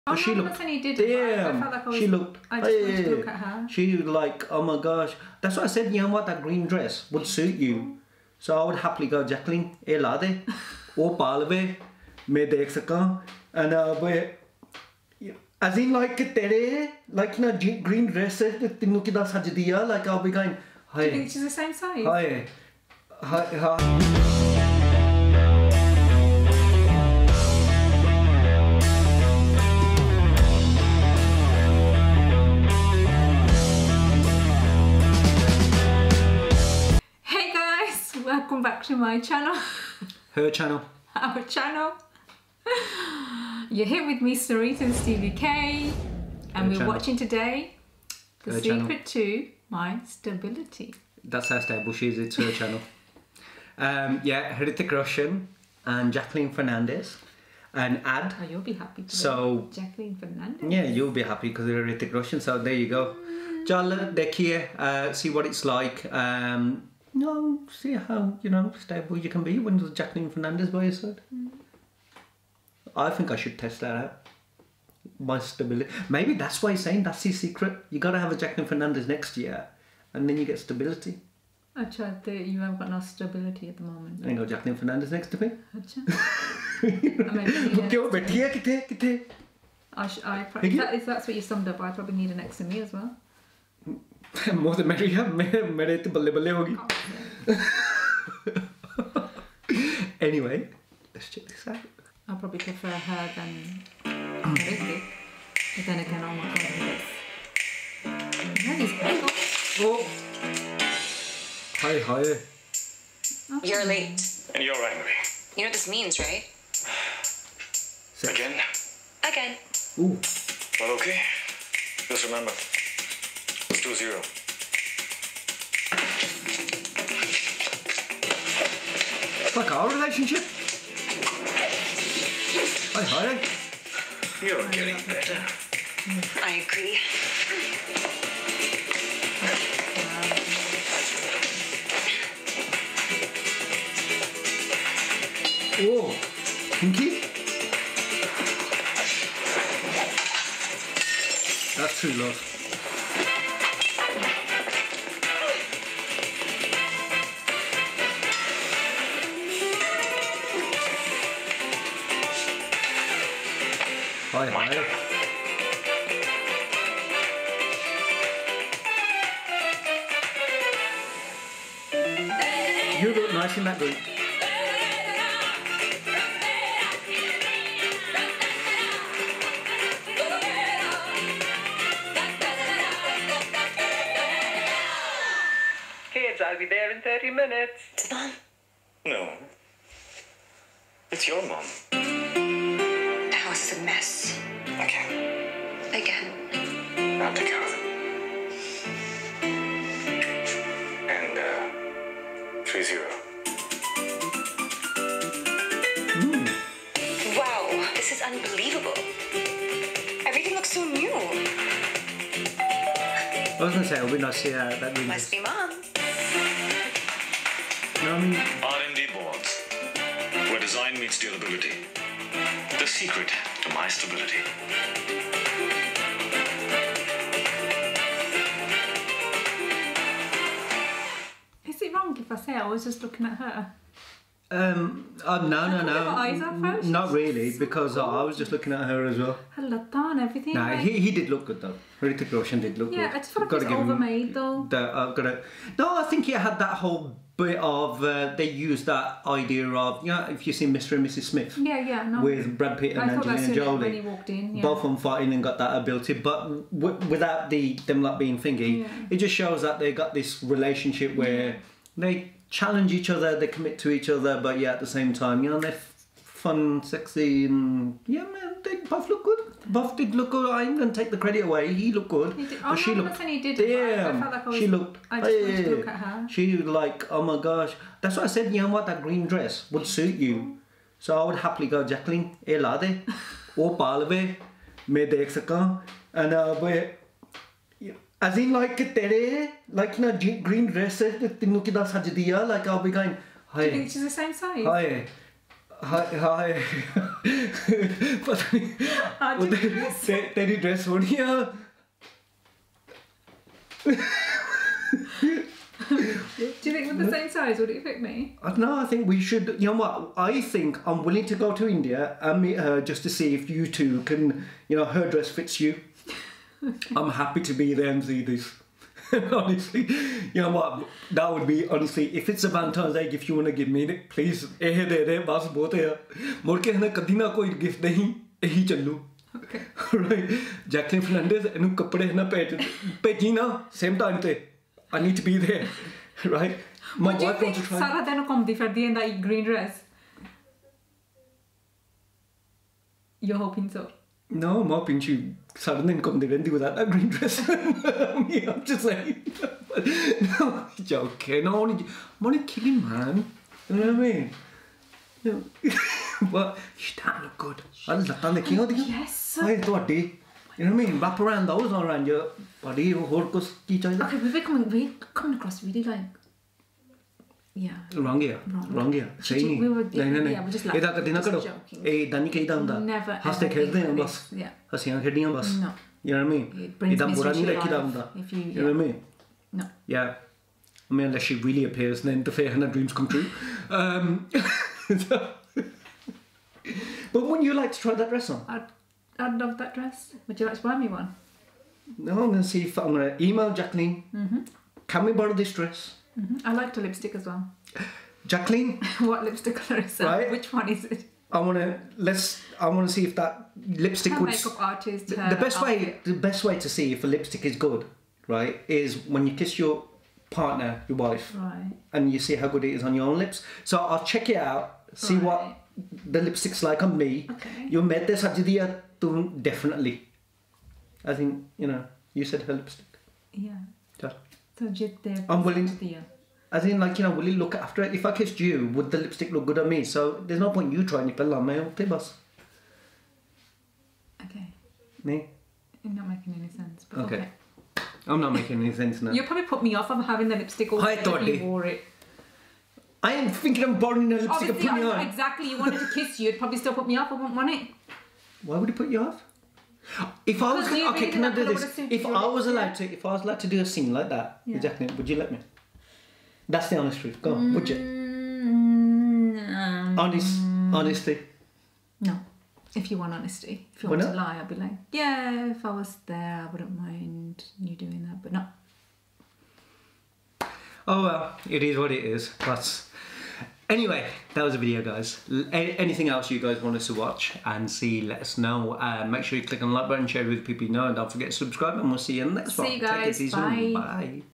'THE SECRET TO MY STABILITY' | Hrithik Roshan | Jacqueline Fernandez | REACTION!!
Hey Guys, here is our HONEST Reaction and Review to Hrithik Roshan and Jacqueline Fernandez in The Secret To My Stability, an Ad Film for RMD Boards.